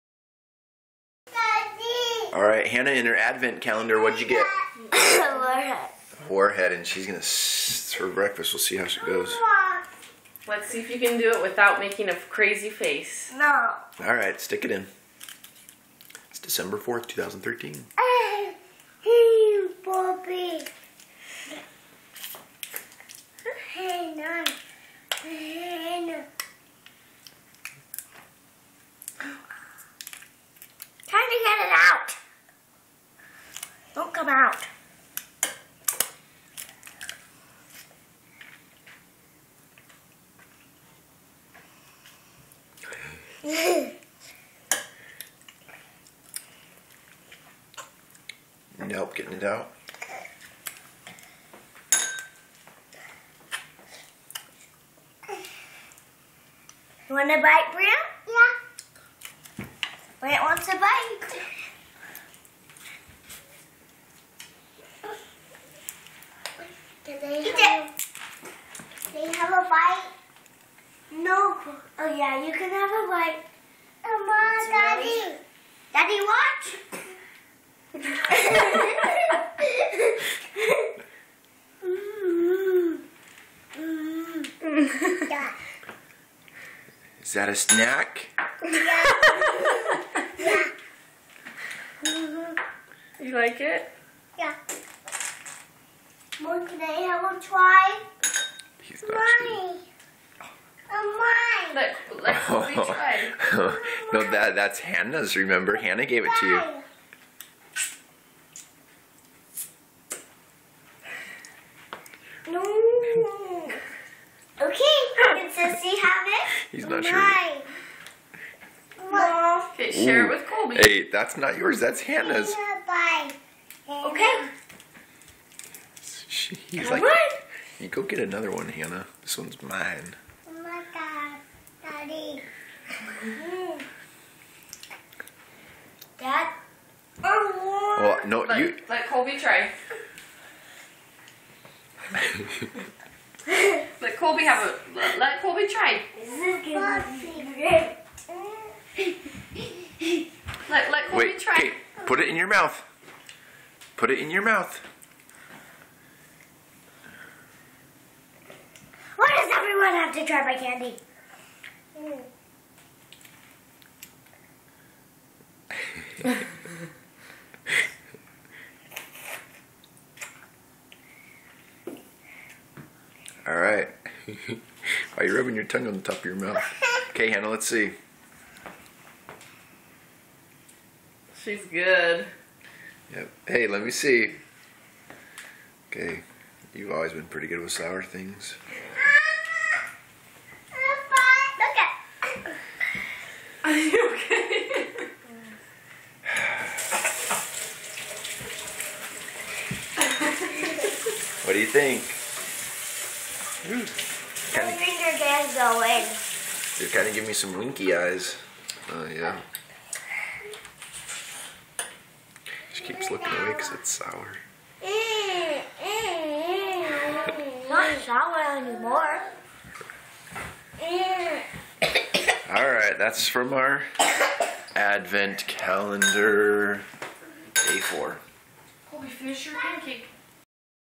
Hannah eating her daily advent calendar item
Hannah got a warhead from her advent calendar today 4 Dec 2013. I wanted a squirrelly face but she rulos a great job. Brant on the other hand was hilarious!